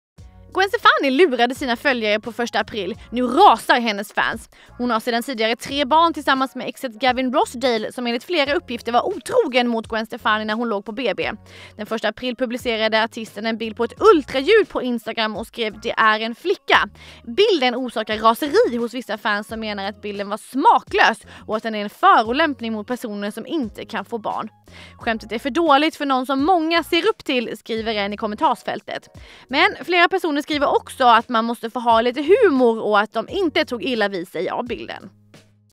Gwen Stefanis ultraljudsbild får fansen att rasa
Därför upprörs följarna av stjärnans gravidbild på Instagram